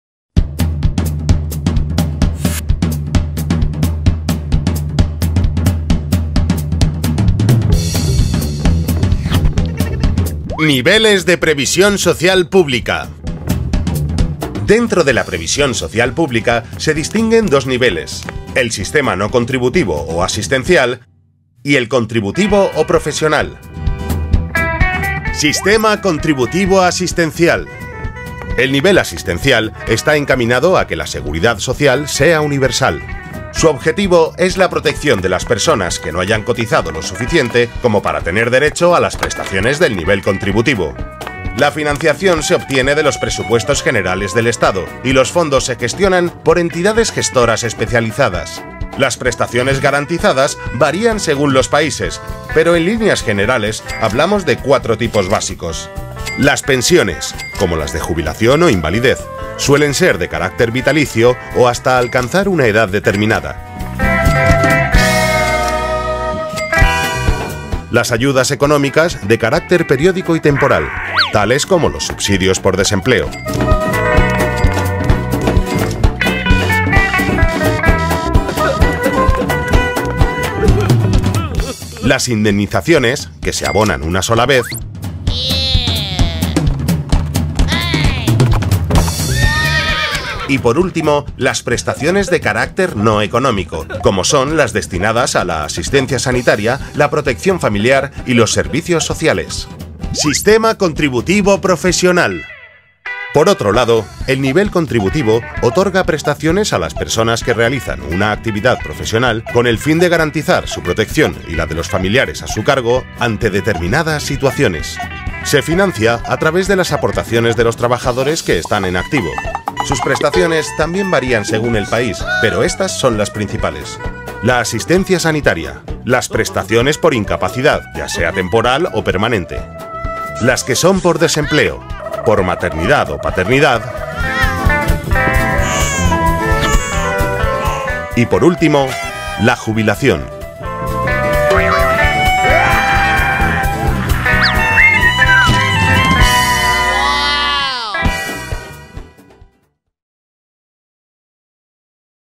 Máximo Pupas. Levels of State Social Welfare – Seguros y Pensiones para Todos
There are two levels within the State Social Welfare system: the non-contributory or welfare system and the contributory or professional system. The goal is for us all to receive a benefit, the one that best fits our circumstances. With Máximo Pupas you can discover the types of benefits that exist in Spain.